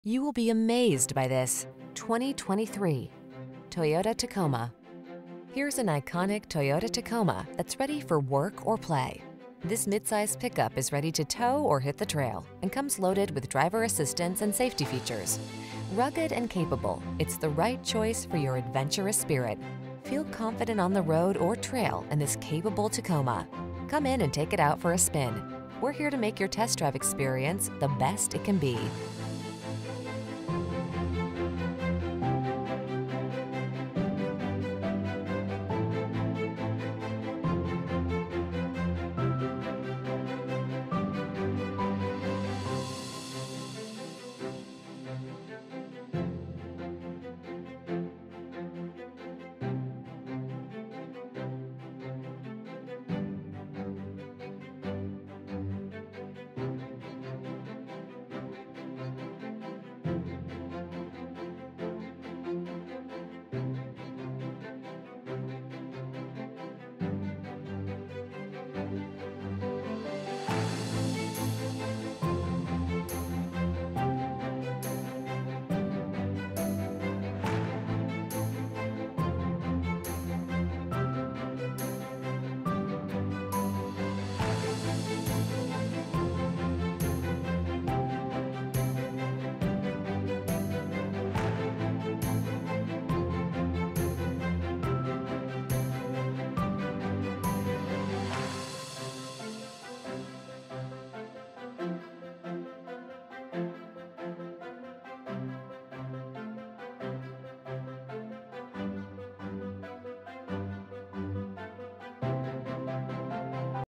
2023 Toyota Tacoma 4WD Westminster, Costa Mesa, Garden Grove, Long Beach, Huntington Beach, CA 21068
3TMDZ5BNXPM157085 Magnetic Gray Metallic New 2023 Toyota Tacoma 4WD TRD Sport available in Huntington Beach, California at Toyota of Huntington Beach. Servicing the Costa Mesa, Garden Grove, Long Beach, Huntington Beach, CA area.